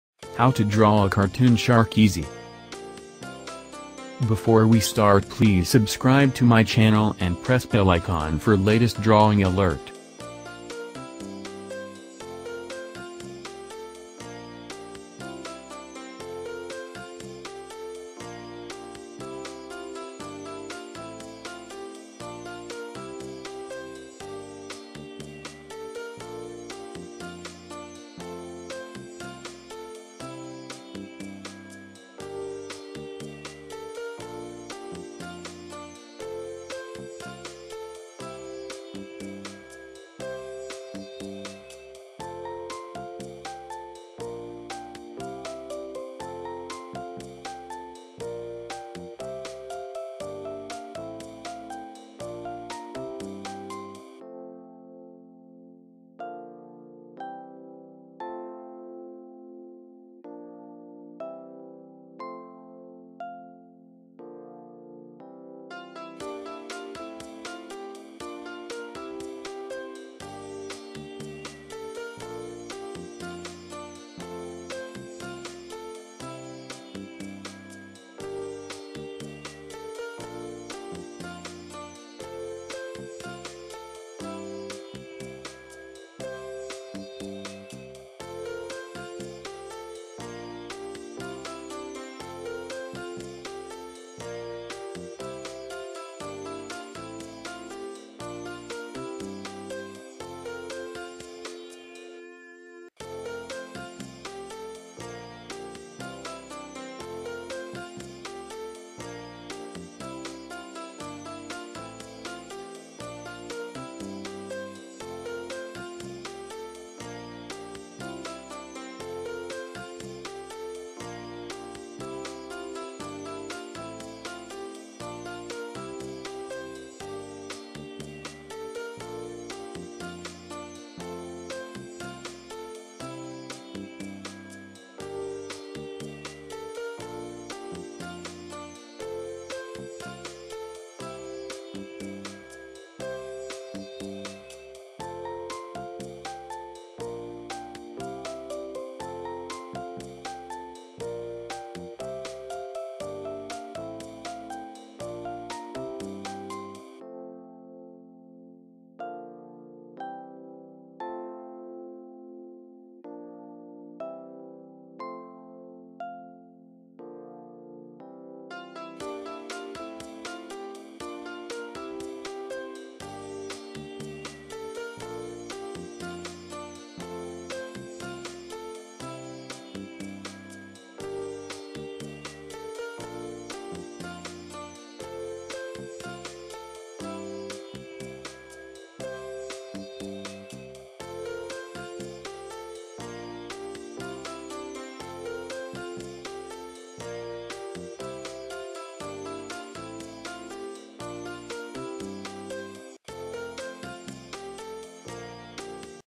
How to draw a Cartoon Shark Easy
If you want to make this video as how to draw a cartoon shark step by step then simply slow down the speed of this tutorial.

I always attracted towards the drawing of cartoon shark because of that heavy head and those sparkling sharp teeth. I tried my level best to draw it and I am sure you guys are going to love it.

Best Amazon Buying links for Drawing Tools -

Best Pencil for Drawing
Best Eraser for Drawing
Best Marker for Drawing